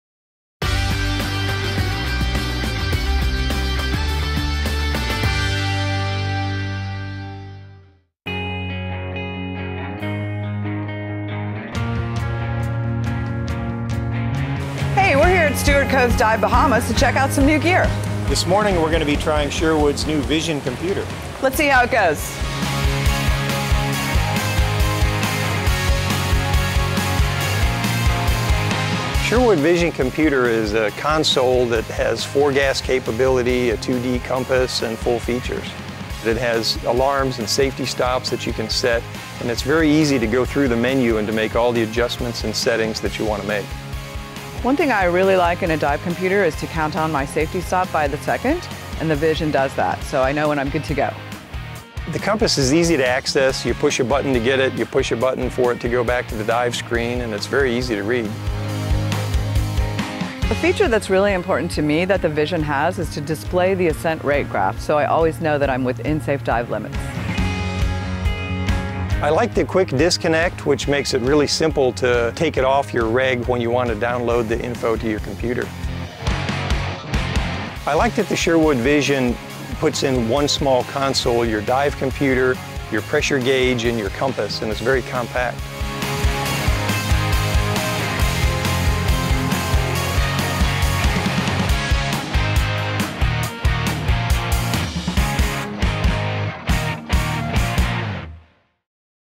Sherwood Scuba's Vision Computer
Editors try Sherwood Scuba's vision computer while diving with Stuart Cove's Dive Bahamas. See what they liked about this console computer.